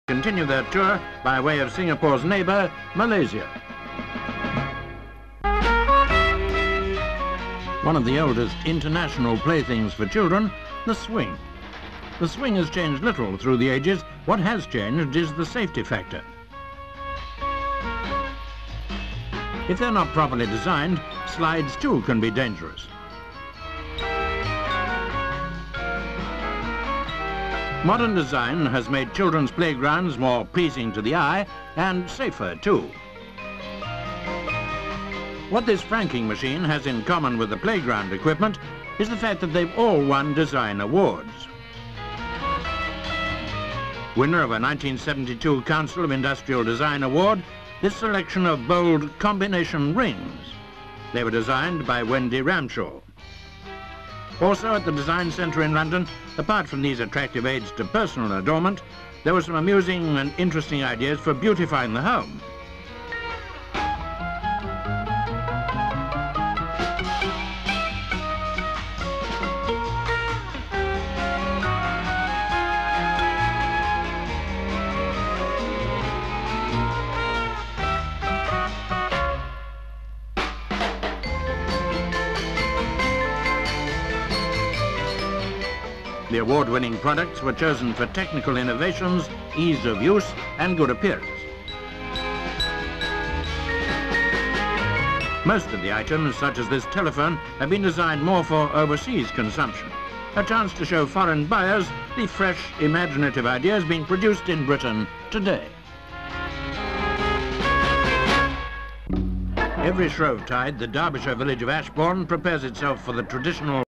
DESIGN FOR TODAY
(28 Feb 1972) At the Design Centre in London awards are given to designers for their modern creations of telephones, furnishings and personal adornments.